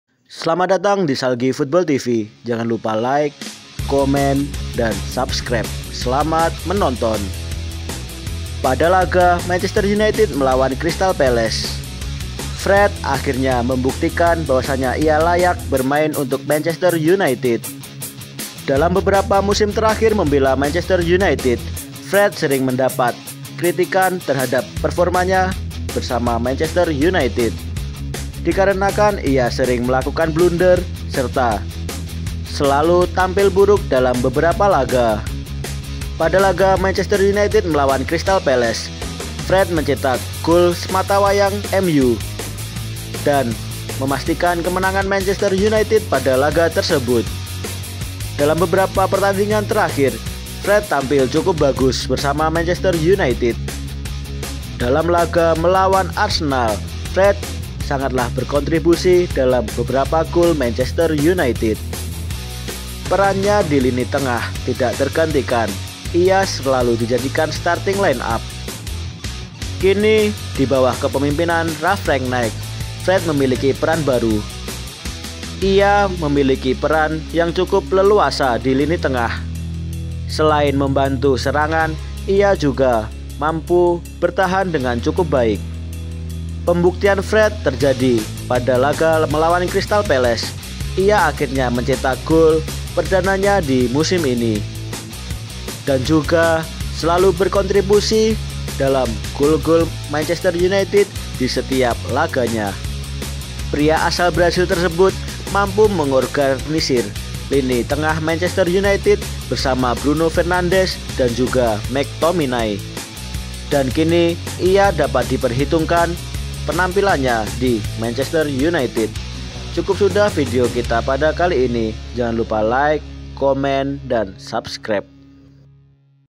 JADI MAN OF THE MATCH, FRED TUNJUKKAN KUALITASNYA
pada laga Manchester United vs Crystal Palace, Fred berhasil menjadi Man of the match pada laga tersebut. ini merupakan pembuktian Fred terhadap keraguan publik.